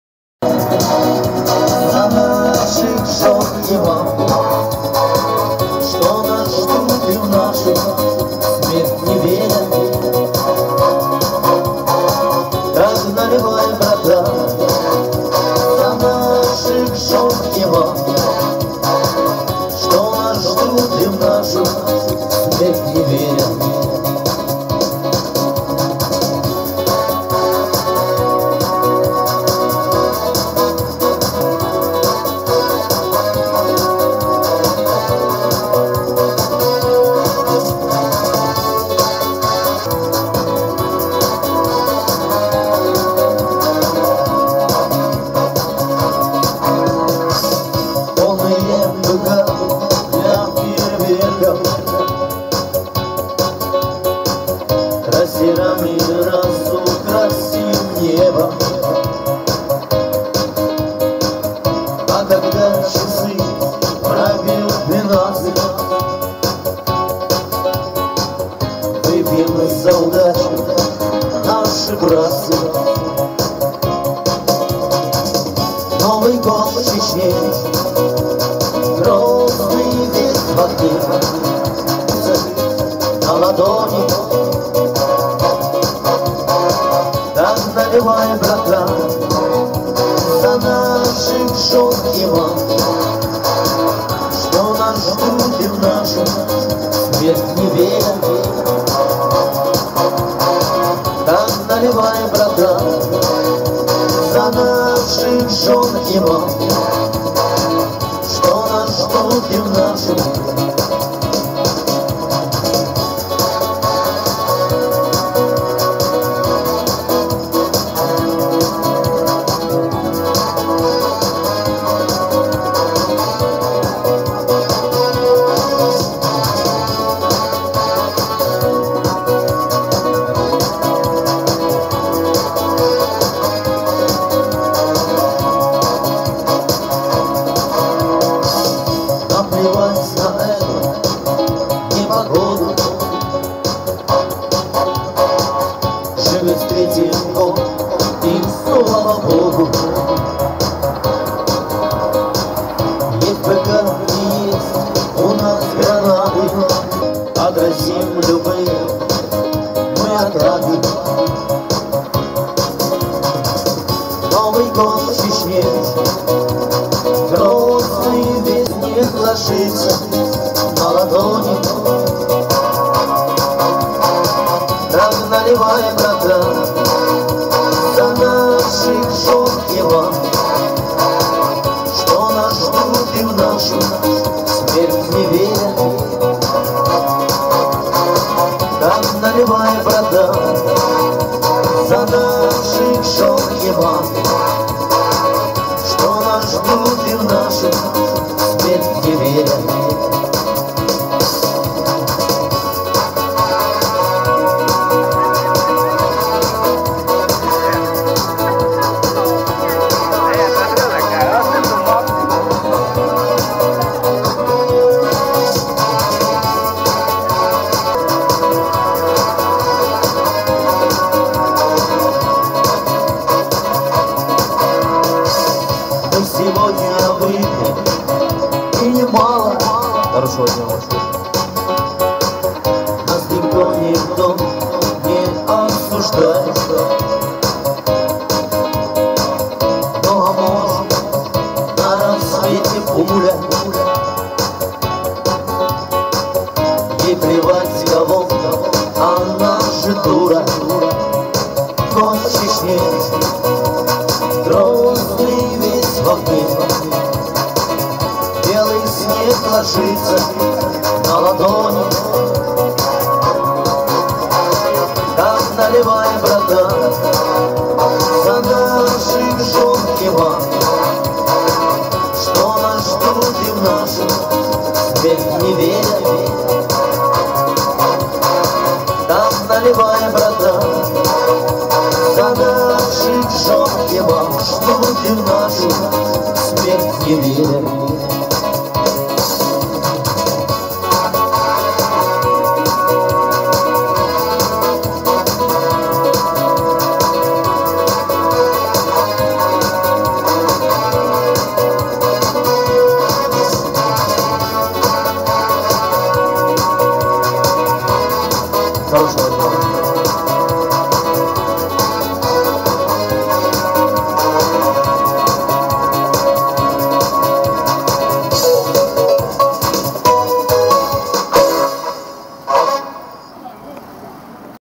Группа Перевал
акция: сбор средств в фонд помощи инвалидам и ветеранам боевых действий г.гусь-хрустальный выступление музыкантов гр. перевал